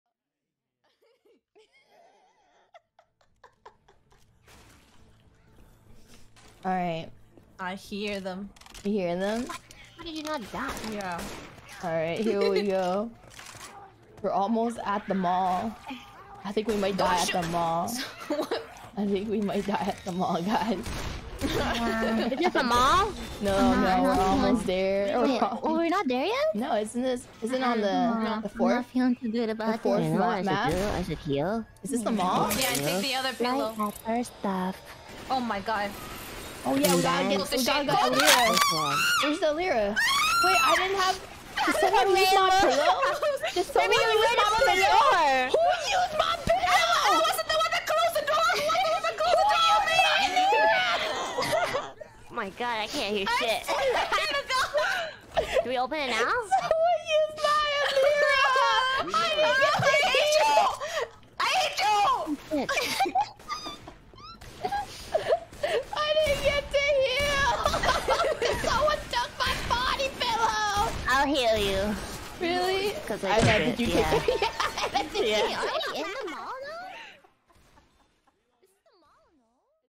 [NijiEN] Someone takes Selen's body pillow
and she gets all torn up about it.